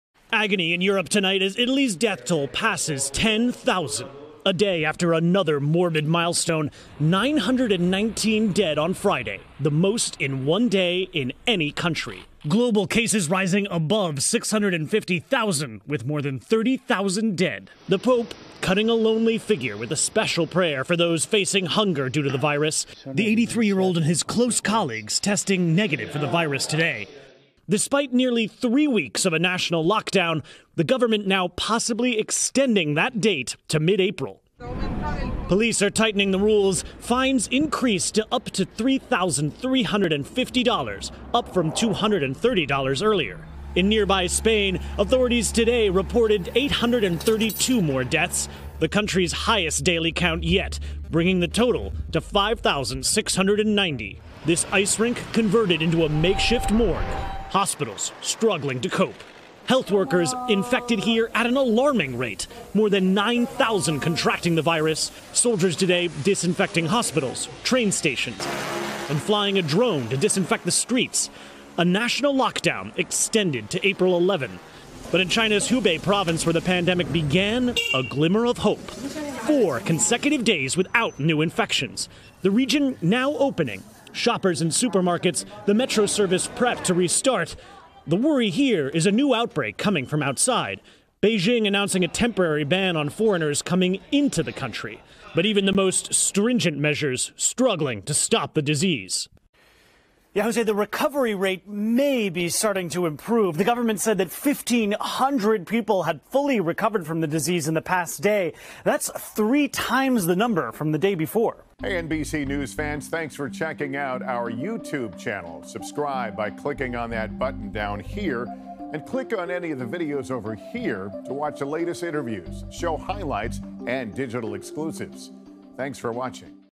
Coronavirus: Fahamu kwanini idadi ya vifo vya Corona COVID-19 viko juu nchini Italy?
More Than 10,000 Dead In Italy From Coronavirus As Global Cases Top 600,000

Coronavirus: Fahamu kwanini idadi ya vifo vya coronavirus iko juu Italy?

Mlipuko wa virusi vya corona unaendelea kuliathiri pakubwa taifa la Italy, huku hospitali zikishindwa kumudu idadi ya maambukizi mbali na kuwekwa kwa sheria ya kutotoka nje.

Lakini wataalam wana wasiwasi kuhusu kiwango cha juu cha vifo huku idadi ya watu waliofariki ikiipiku ile ya China.

Kati ya visa 86,500 vya maambukizi vilivyothibitishwa nchini Italy na  Zaidi ya watu 10,000 wamefariki .